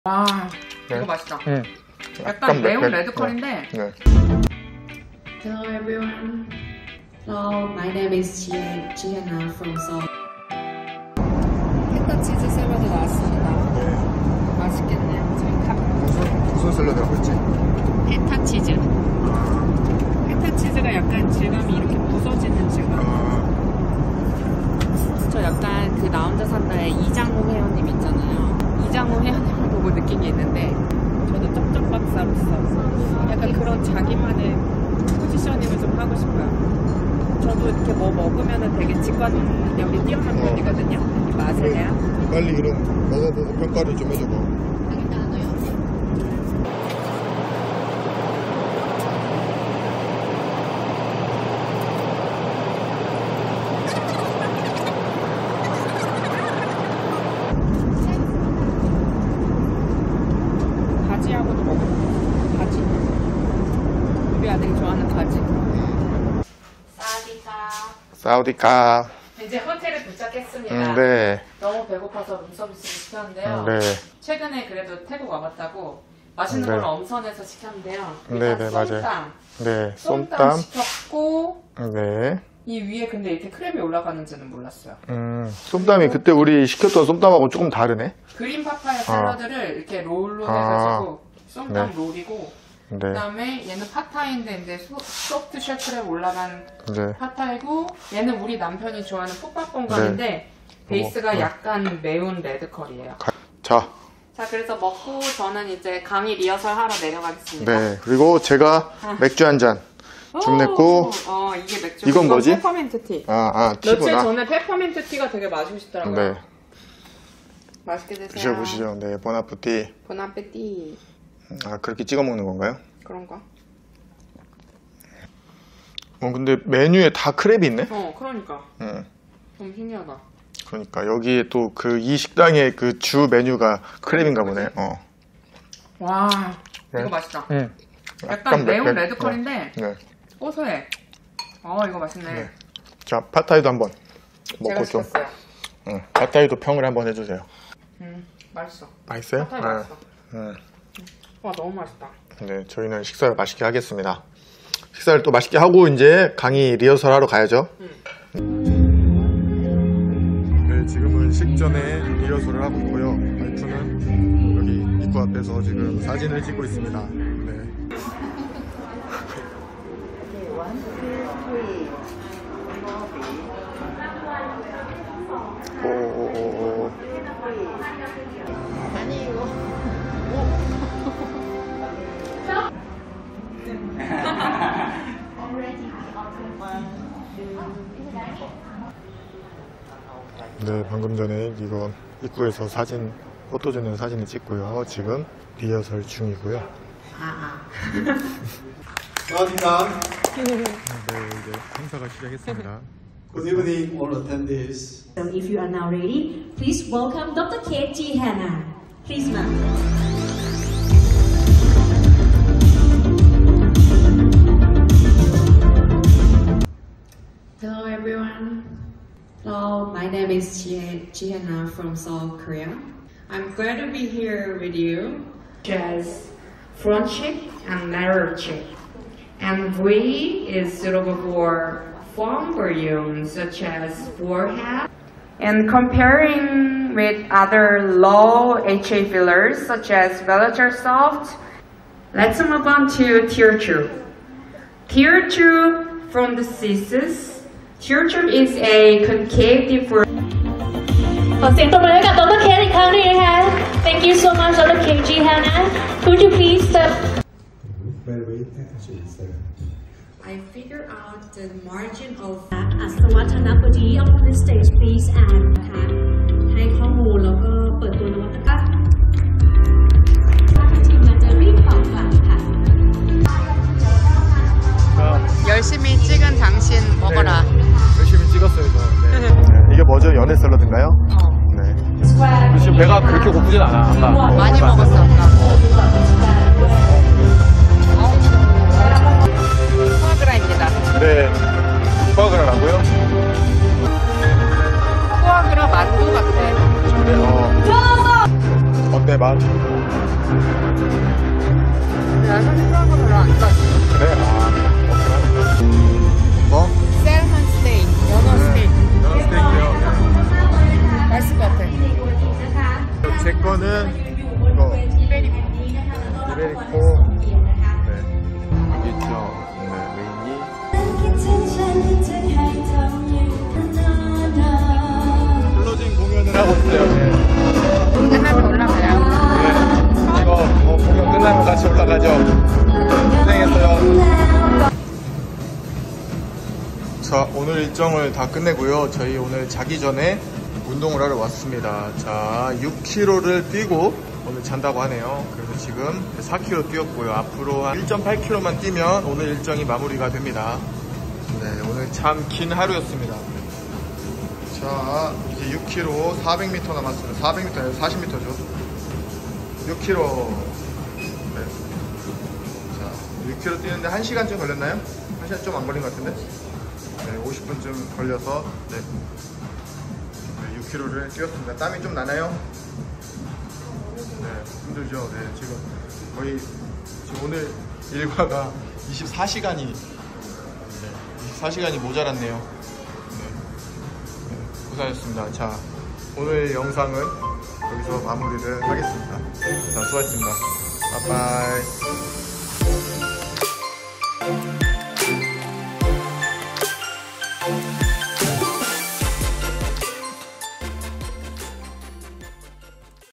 K뷰티를 세계에 알리러 go go | 차메디텍 하이필리아 런칭 행사
출장 첫째날, 새벽 2시에 일어나 집에서 출발해서 6시반쯤 인천공항에 도착했습니다. 라운지에서 아침을 간단히 먹고, 조금 쉬다가 비행기를 탔습니다. 이륙하자마자 기내식이 나왔는데 의사엄마모델아빠는 사전예약메뉴를 주문했습니다. 먼저 통새우타르트가 나왔습니다. 다음으로 페타치즈샐러드, 메인요리로 의사엄마의 광어구이와 모델아빠의 닭고기구이가 나왔습니다. 6시간의 비행 후 드디어 방콕에 도착해서
오후쯤 호텔에 도착했는데 배가 너무 고파 타이음식으로 룸서비스를 시켰습니다.
잠시 쉬다가 학회장으로 항했습니다. 의사엄마는 행사장 포토존에서 사진 촬영을 하고 강 리허설을 했습니다. 리허설이 끝나고 차메디텍 회사에서 만든 하이필리아라는 필러 런칭 행사가 바로 시작되었습니다.
처음으로 차메디텍 대표이사님의 인사와 개회사가 있었습니다.
이어서 의사엄마의 하이필리아 필러 강의가 있었습니다.
의사엄마는 한국을 대표해서 방콕 의사들과 인플루언서들에게 하이필리아 필러의 사용법과 장단점에 대해 설명해 주었습니다.
의사엄마 강의를 마치고 시상식과 함께 코스식사를 즐겼습니다.
행사 클로징공연 관람을 마치고 숙소에 들어오자마자 운동복으로 갈아입고  피트니스센터에 가서 운동을 하였습니다. 기상한지 24시간이 넘었지만 강의도 하고 운동까지 한 의사엄마.. 대단하네요.